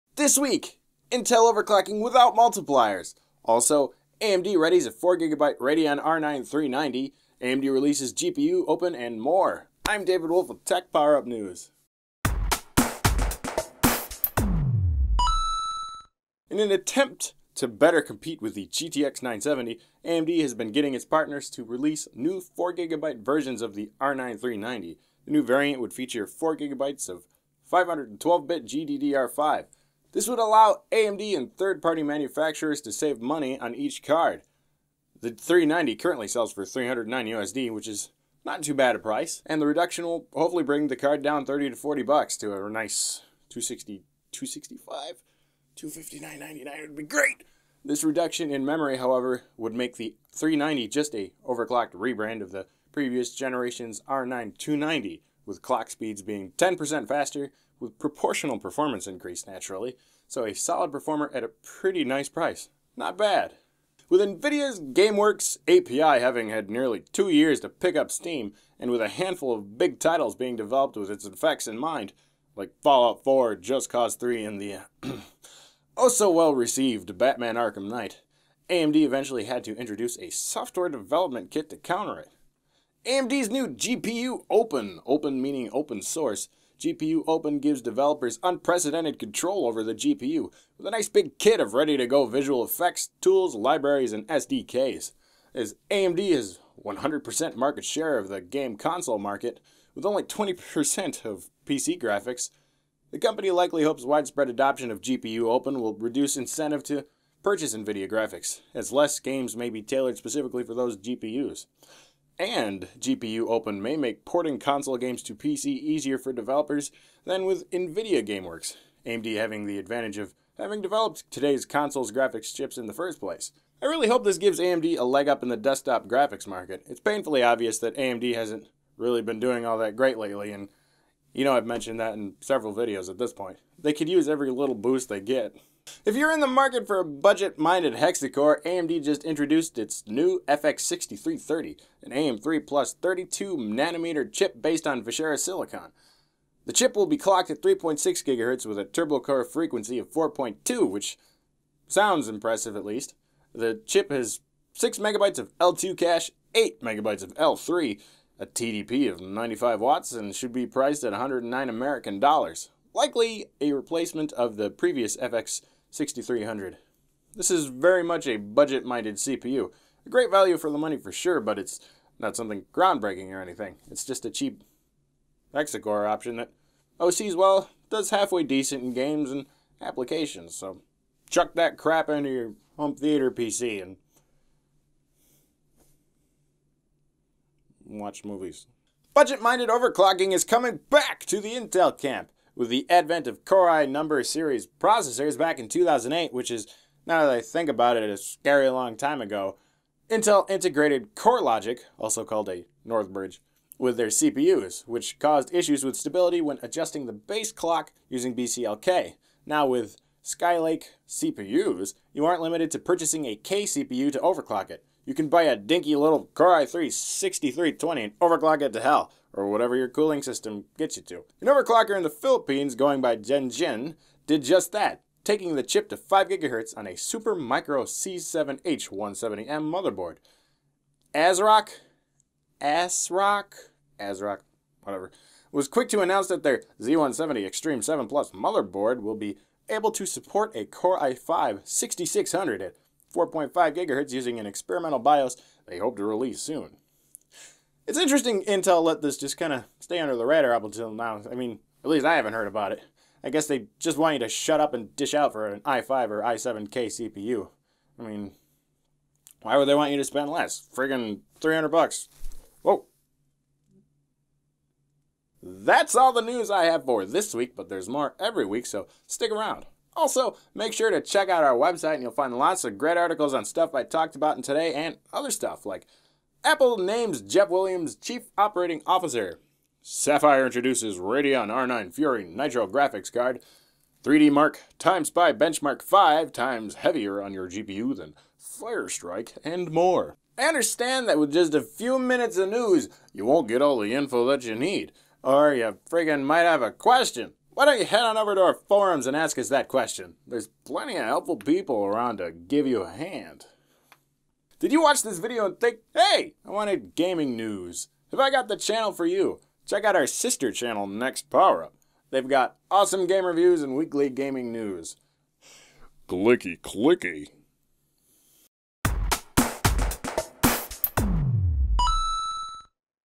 TechPowerUp! News : Return of Budget Intel Overclocking, AMD GPUOpen
TechPowerUp! News : Return of Budget Intel Overclocking, AMD GPUOpen, 4GB Radeon R9 390, and more!

AMD Readies 4 GB Variant of the Radeon R9 390

Rejoice! Base Clock Overclocking to Make a Comeback with "Skylake"

Wanna read more tech news? Hit up